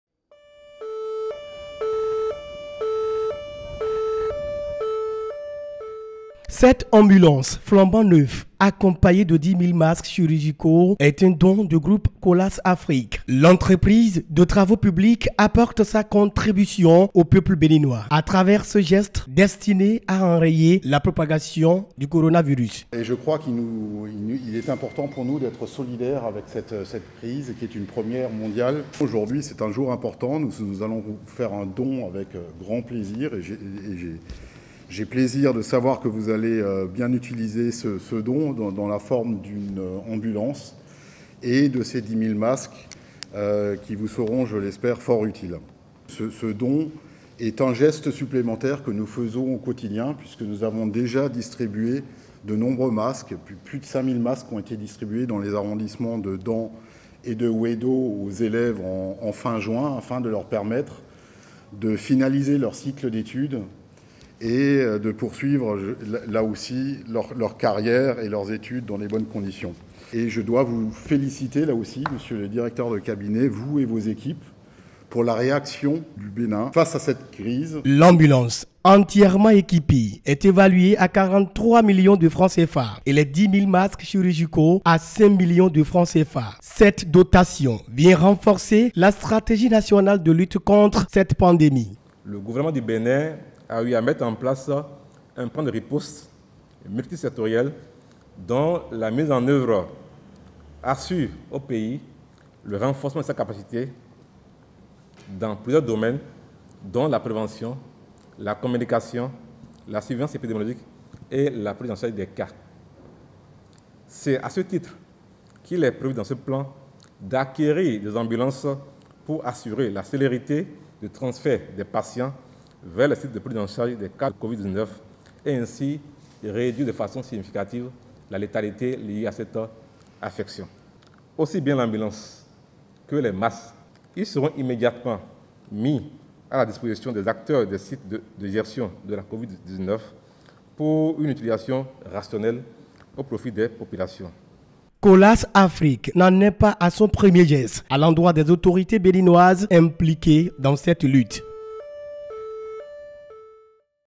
Secourisme : le groupe Colas Afrique fait un don d’une ambulance au Bénin
Le groupe Colas Afrique offre au Bénin  une ambulance et 10 milles masques chirurgicaux.  A travers ce geste, le groupe veut accompagner le gouvernement dans la lutte contre la pandémie de coronavirus.